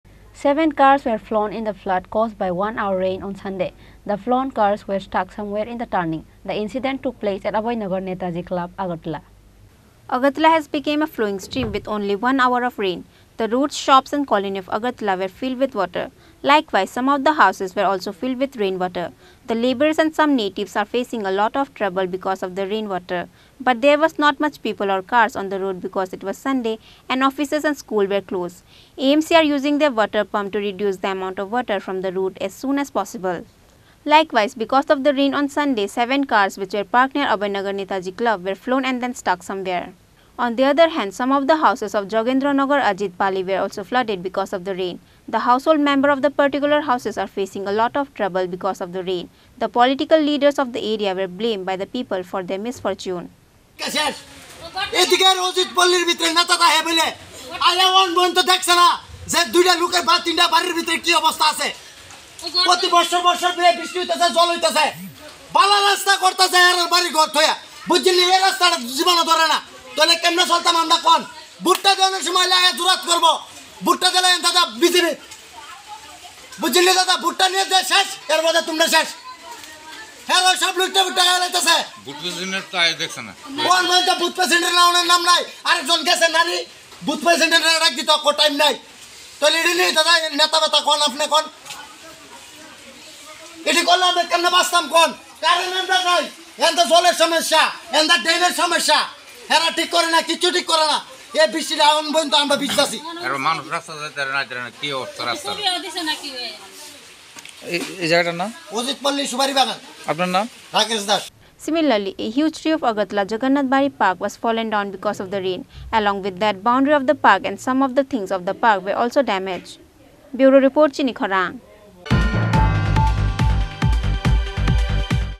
Seven cars from Netaji Club Abhoynagar swept away in the flood on Sunday
🟢 Seven cars were swept away in the flood caused by 1 hour rain on Sunday. The flown cars were stuck somewhere in the turning. The Incident took place at Abhoynagar Netaji Club, Agartala.